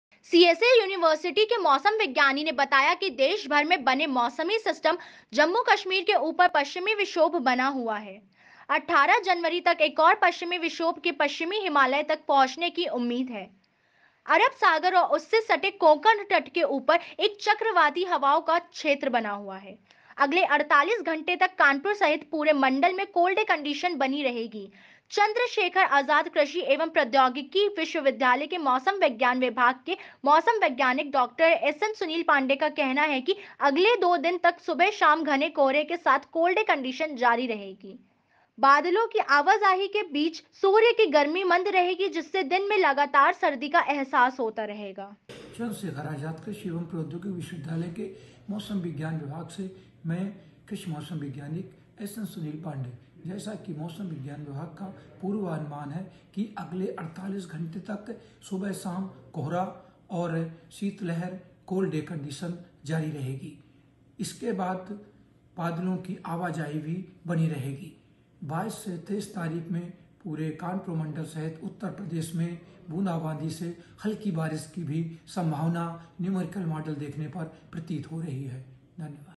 Kanpur : सी॰एस॰ए॰ यूनिवर्सिटी के मौसम विज्ञानी ने बताया की देश भर में बने मौसमी सिस्टम || abnews
@ABNEWS INDIA
18 जनवरी तक एक और पश्चिमी विक्षोभ के पश्चिमी हिमालय तक पहुंचने की उम्मीद है